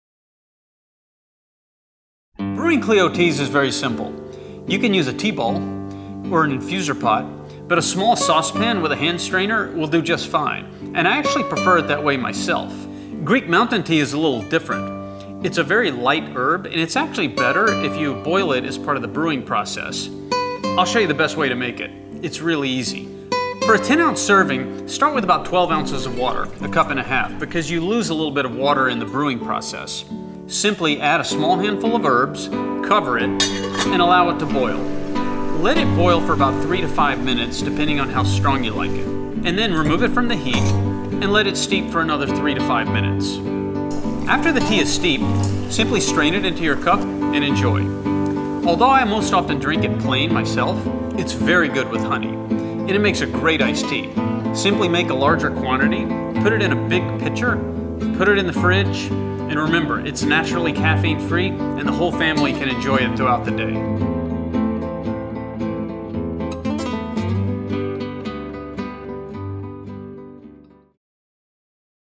Brewing Greek Mountain Tea | Klio Tea
How to brew Klio Organic Greek Mountain Tea - a delicious herbal infusion, rich in antioxidants and phyto-nutrients.